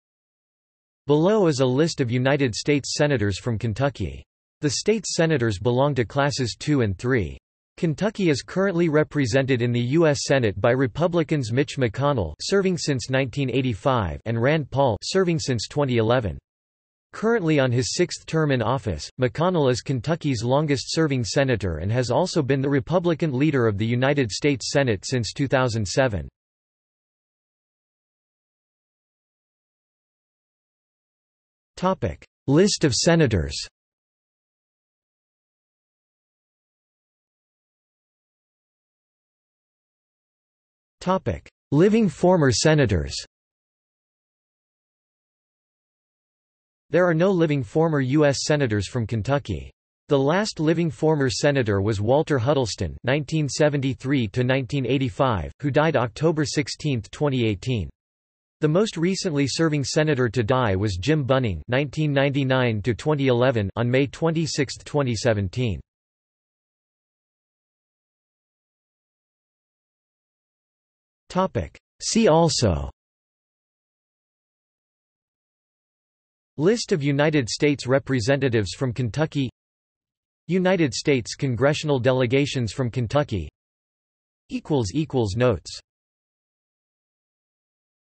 List of United States Senators from Kentucky | Wikipedia audio article
This is an audio version of the Wikipedia Article:
List of United States Senators from Kentucky

00:00:28 1 List of Senators
00:00:37 2 Living former senators
00:01:07 3 See also

- Socrates

SUMMARY
Below is a list of United States Senators from Kentucky. The state's Senators belong to Classes 2 and 3. Kentucky is currently represented in the U.S. Senate by Republicans Mitch McConnell (serving since 1985) and Rand Paul (serving since 2011). Currently on his sixth term in office, McConnell is Kentucky's longest serving Senator and has also been the Republican Leader of the United States Senate since 2007.